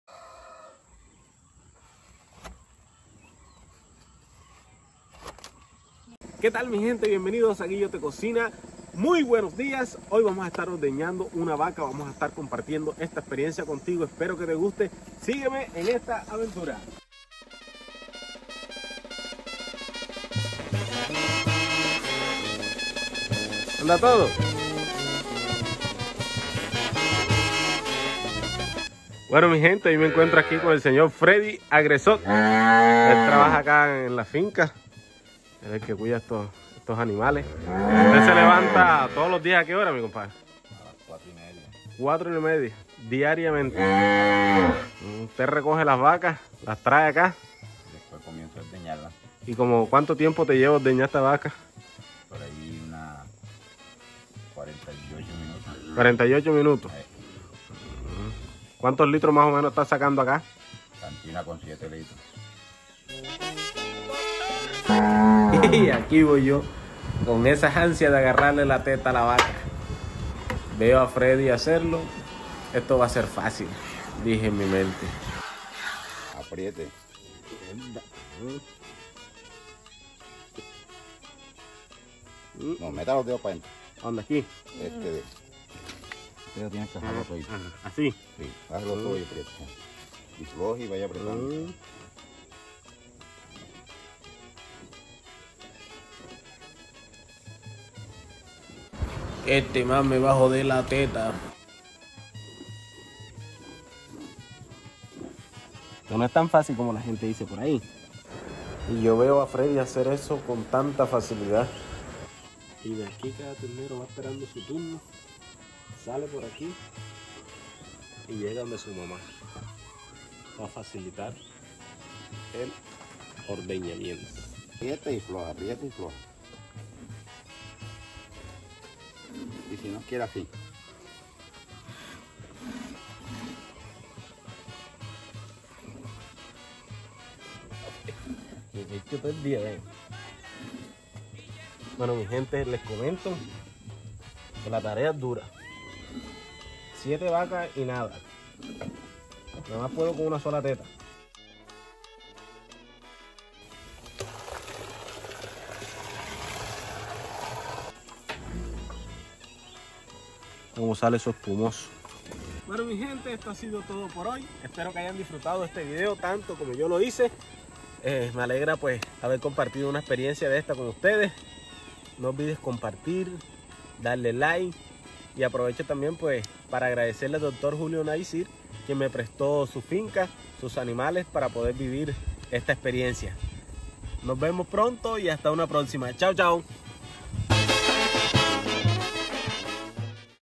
Como ordeñar una vaca
Mi experiencia tratando de ordeñar una vaca por primera vez 💪🏽